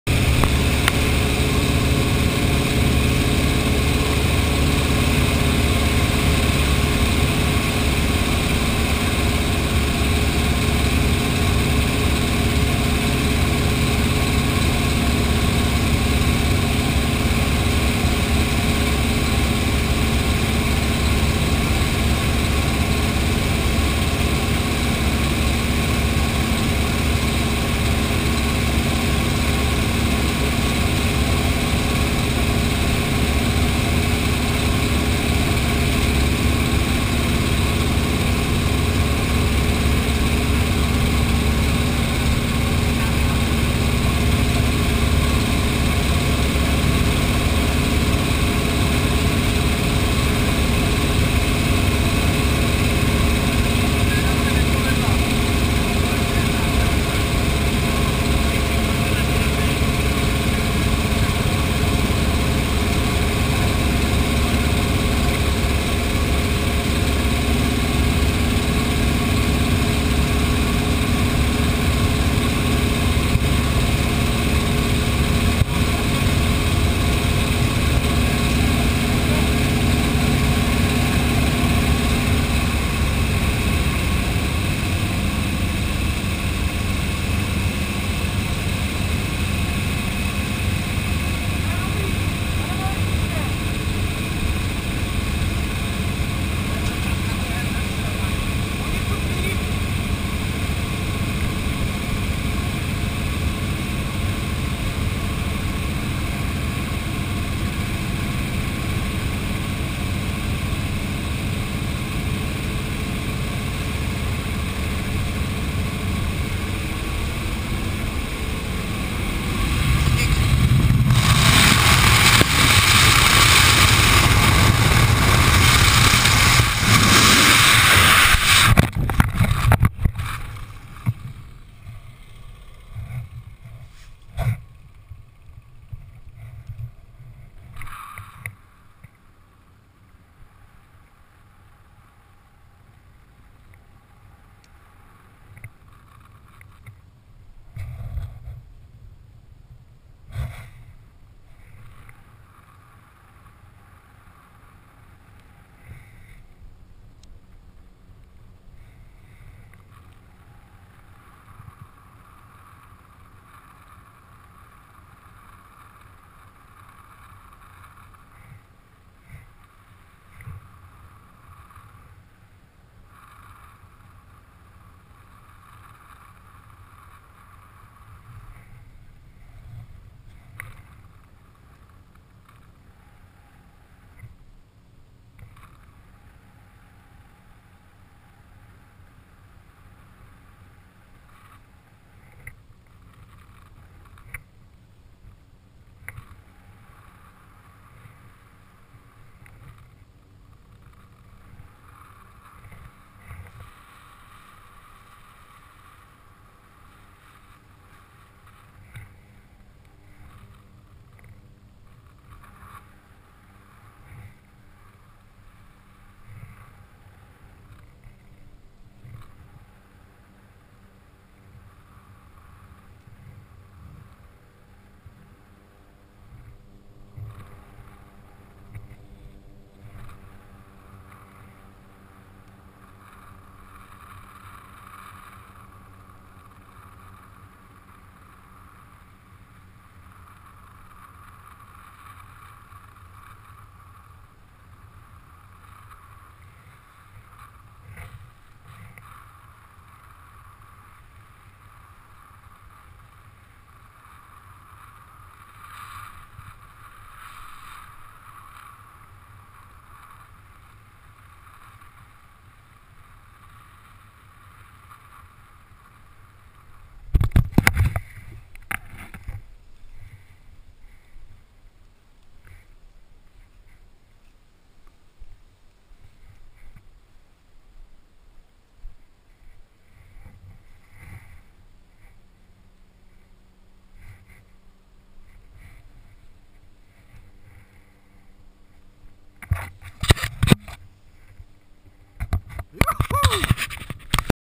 Round parachute jump
My 304th jump, this time on a round parachute OVP-68. Designed in 1968, now used in 2010 :-) I did it as a static line jump in Hořovice, Czech Republic, from AN-2 plane ("Andula") from 700m above terrain.